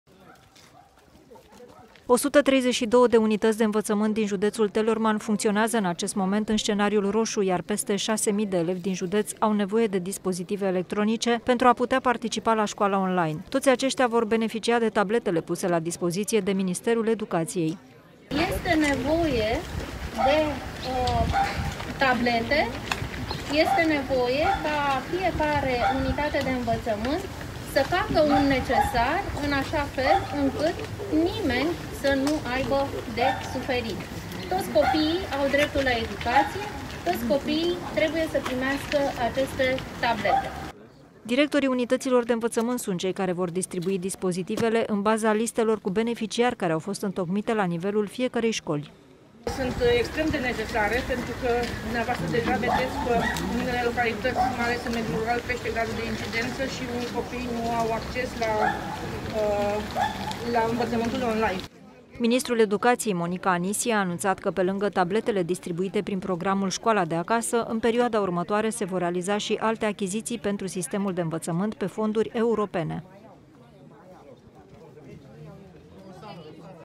AU SOSIT TABLETELE
Peste 6.000 de elevi din Teleorman vor avea, începând de astăzi, tabletele necesare pentru desfășurarea cursurilor în sistem on-line. Dispozitivele, puse la dispoziție de Ministerul Educației, au ajuns, în această dimineață, în județ iar recepția acestora s-a făcut în prezența ministrului Monica Anisie.